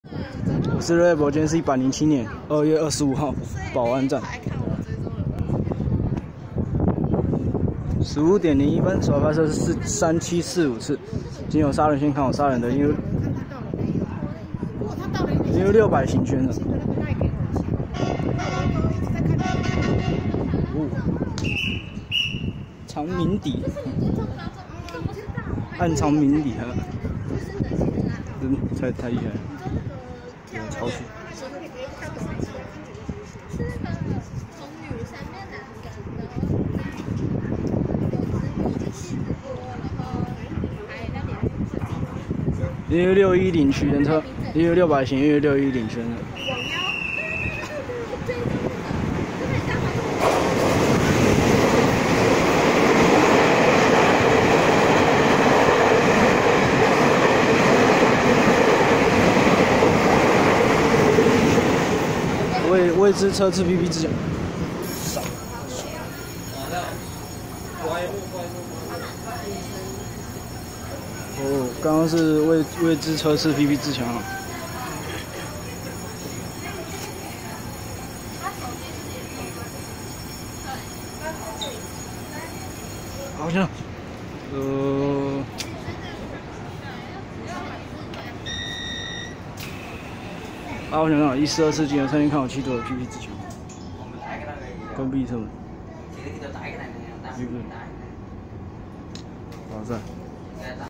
3745次EMU600型EMU610區間車，跟134次PP自強號
107/2/25 保安站 15:01拍攝的3745次經由沙崙線開往沙崙的EMU600型EMU610區間車，跟134次經由山線開往七堵的PP自強號